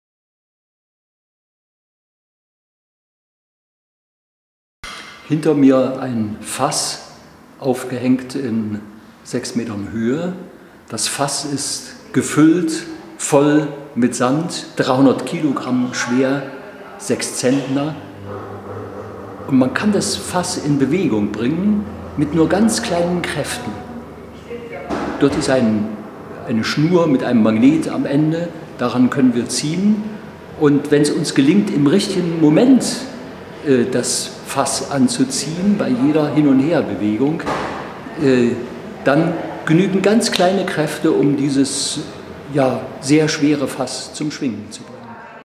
Das pendelnde 300-Kilogramm-Fass
Es braucht keinen großen Kraftaufwand, um ein 300 Kilogramm schweres Fass in Bewegung zu setzen.